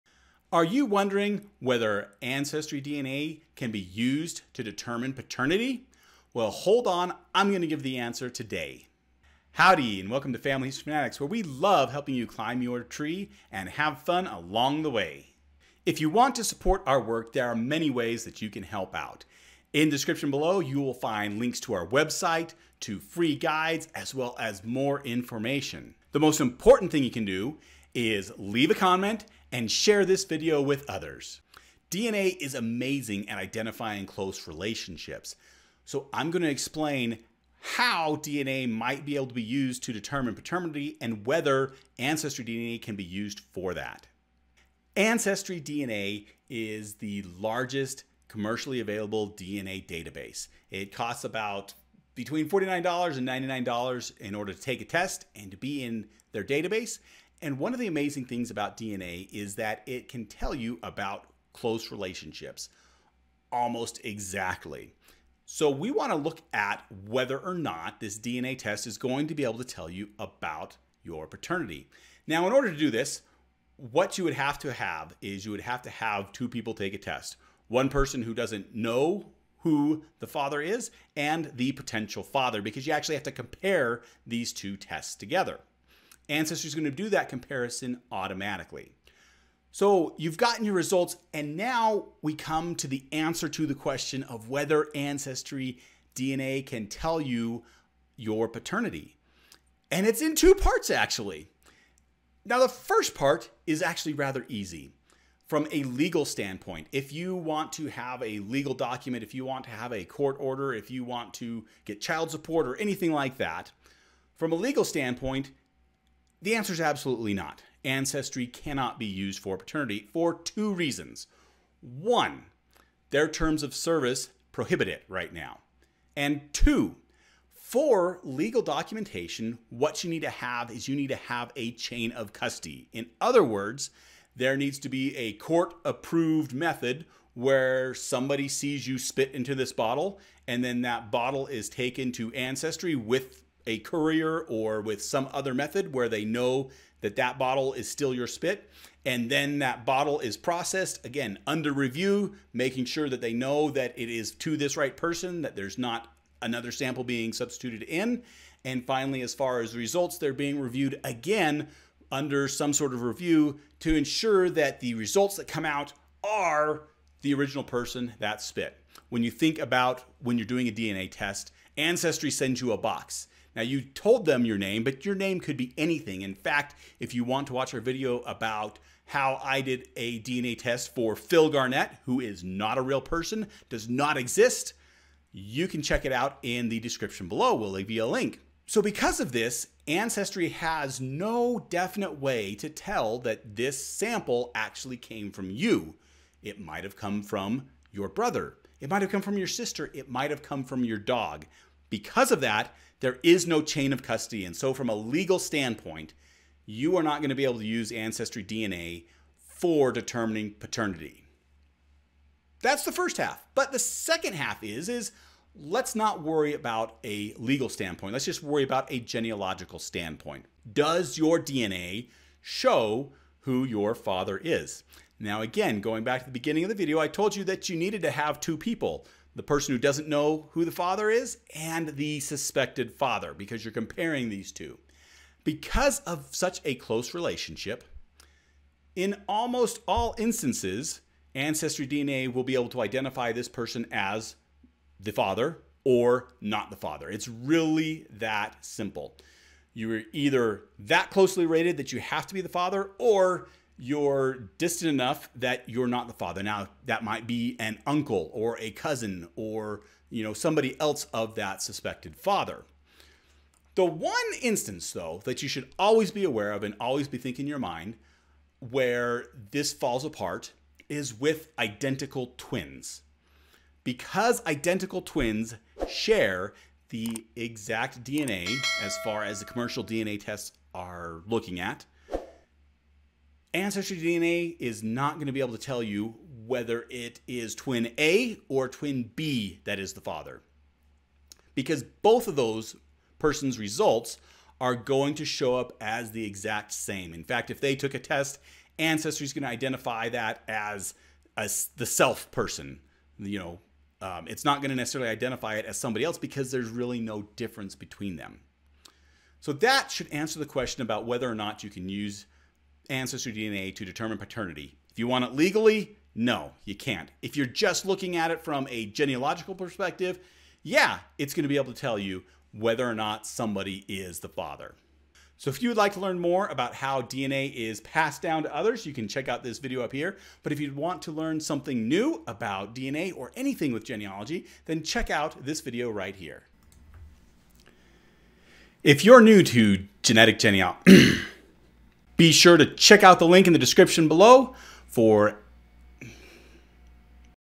Can an Ancestry DNA test prove paternity? | Genetic Genealogy Explained
Can an AncestryDNA test determine someone's paternity? Discover if AncestryDNA can determine the paternity of an individual from a legal and a genealogical perspective.

CONTINUE LEARNING

CHAPTERS

00:00 Intro
01:57 Can Ancestry be used for paternity?
03:40 Does your DNA show who your father is?
04:31 What about identical twins?